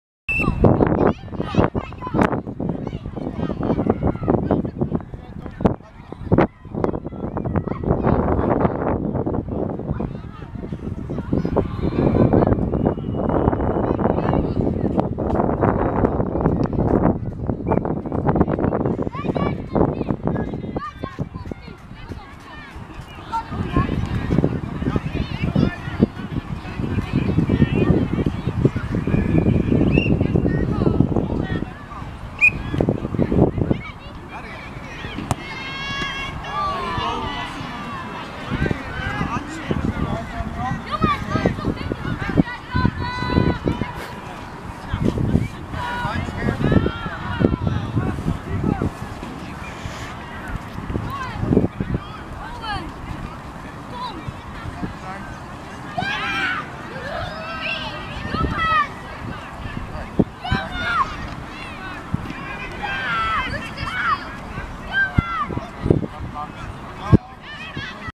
Eindhoven E2 ; Geldrop E3 2 ; 4 018
Strafschoppen Geldrop E3 knokt samen naar een verdiende overwinning